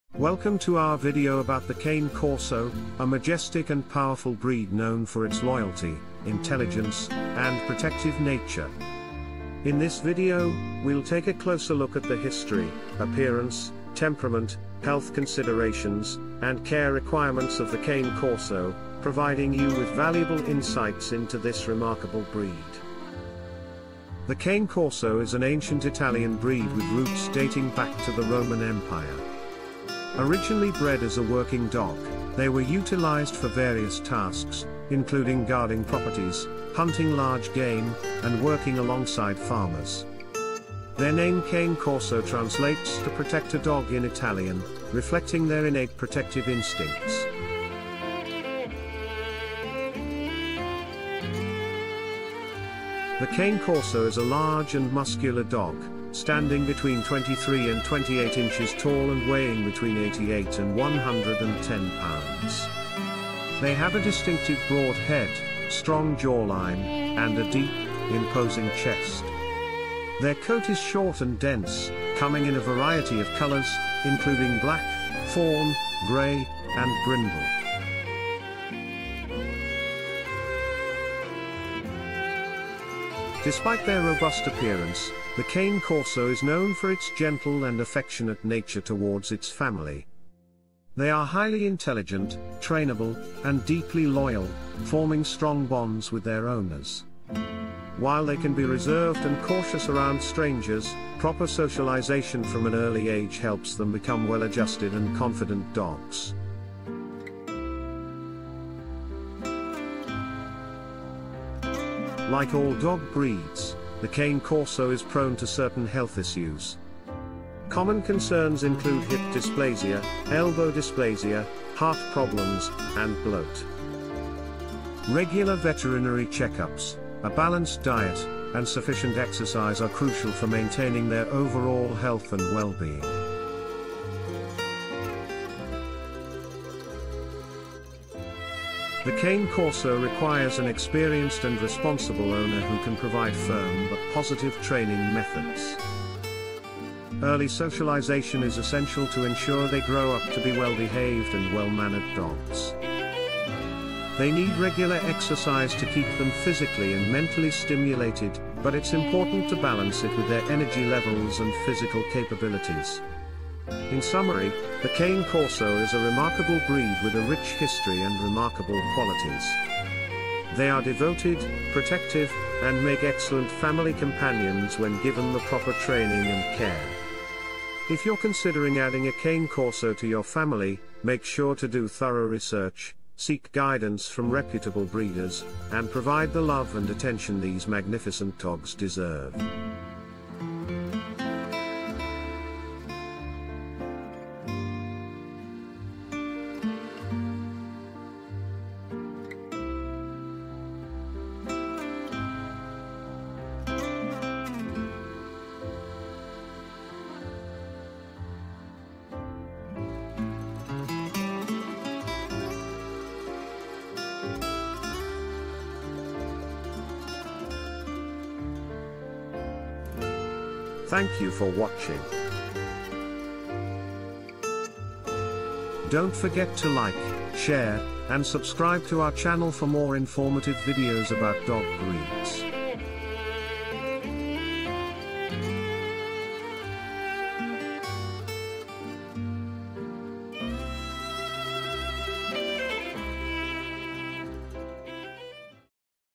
Cane Corso | Unleashing the Power and Loyalty
Welcome to @Breederman, your ultimate destination for dog breed insights! In this video, we'll dive into the captivating world of the Cane Corso, an extraordinary Italian breed known for their strength, loyalty, and protective nature.

Join us as we explore the history, appearance, temperament, health considerations, and care requirements of the Cane Corso. Whether you're a passionate Cane Corso owner or simply fascinated by this remarkable breed, our video offers expert advice, practical tips, and fascinating facts to help you understand and appreciate the true essence of the Cane Corso.